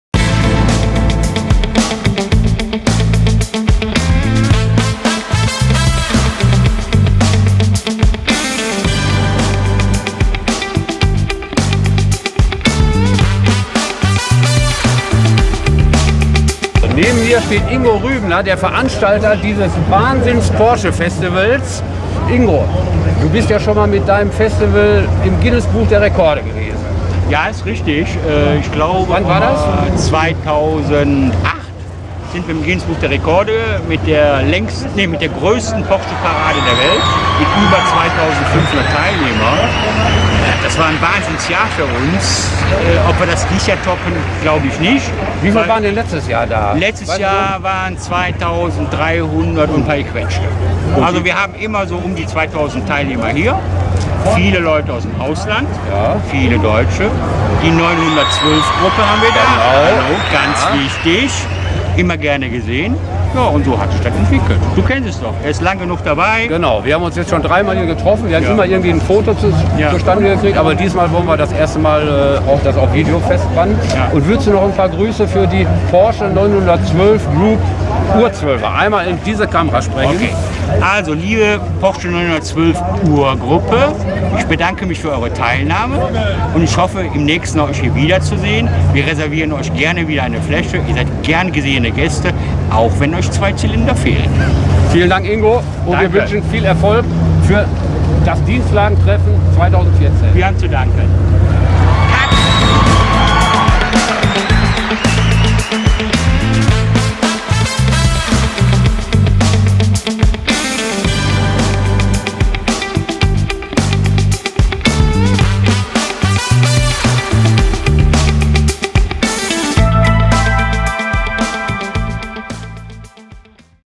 Ingo Rübener grüßt Porsche 912 Group Urzwölfer
Horst und Daggi sind unterwegs ...für die Porsche 912 Group Urzwölfer sprechen wir anlässlich des 13. Intern. Clubday der Porschefreunde 1. Mai auf der Trabrennbahn in Dinslaken
mit Porsche-Urgestein und Veranstalter Ingo Rübener.